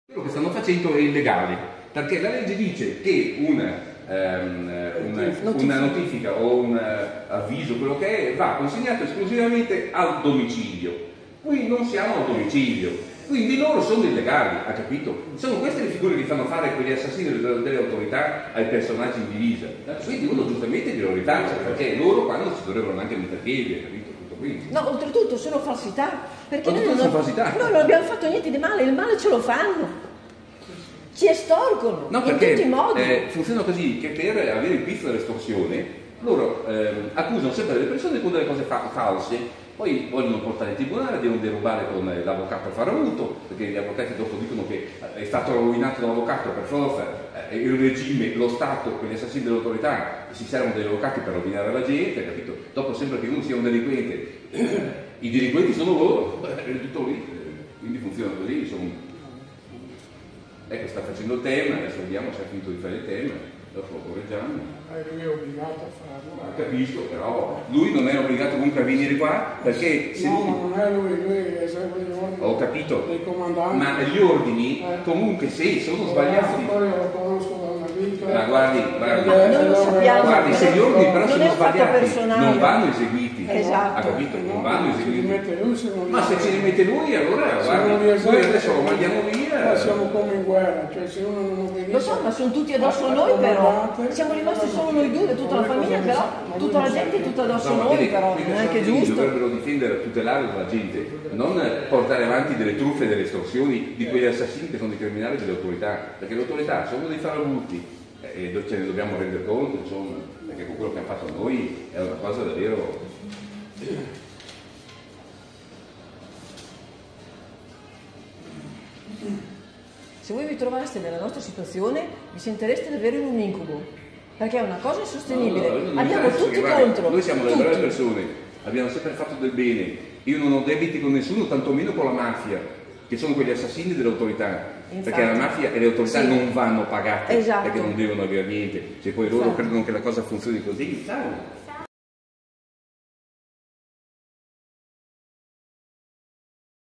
ACCANIMENTO DELLA DITTATURA-MAFIA CONTRO LA FAMIGLIA GANDINI DI FERRARA, PER FINI ESTORSIVI!
PARTE 2 - ANCORA ILLECITI PERPETUATI ATTRAVERSO LE FORZE DELL'ORDINE, CHE SEMINANO TERRORE SU IMPOSIZIONE DELLA DITTATURA-MAFIA (AUTORITÀ-PREFETTURE-POLITICI) PER FINI ESTORSIVI! - Io e mio fratello Alessandro, ci trovavamo a Lido degli Estensi in hotel per alcuni giorni. La nostra famiglia è sotto ESTORSIONI da parte della dittatura-mafia (autorità prefetture politici), e per questo motivo, siamo continuamente pedinati dalle autorità, le quali si servono di tutti: cittadini, istituzioni, forze dell'ordine per compiere illeciti e maltrattamenti contro la nostra famiglia! - Anche in questi pochi giorni di vacanza abbiamo dovuto subire l'intrusione abusiva delle forze dell'ordine in hotel, con motivi assurdi e inconsistenti, ma tali da creare angoscia e accanimento contro la nostra famiglia, come RICATTO PER IGNOBILI FINI ESTORSIVI! 🇮🇹 "LA FAMIGLIA GANDINI DI FERRARA. STORIA ATROCE DI PERSECUZIONE E OMERTÀ!" 🇮🇹